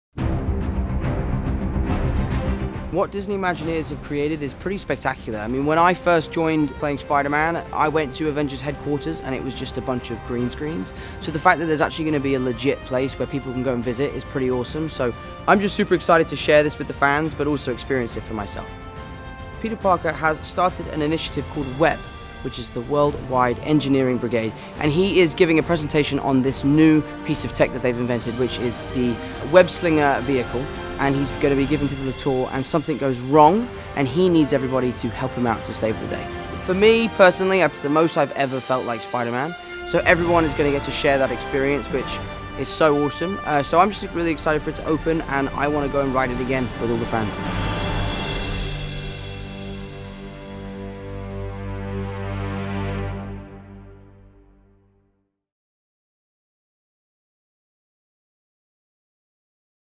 First Look: Tom Holland as Peter Parker in WEB SLINGERS | 3D Glasses | Preshow | Disneyland Resort
disney gave us a new look at Tom Holland recording lines for the preshow of the new Spiderman ride coming to Avengers Campus, plus a look at the AR glasses guests will be wearing!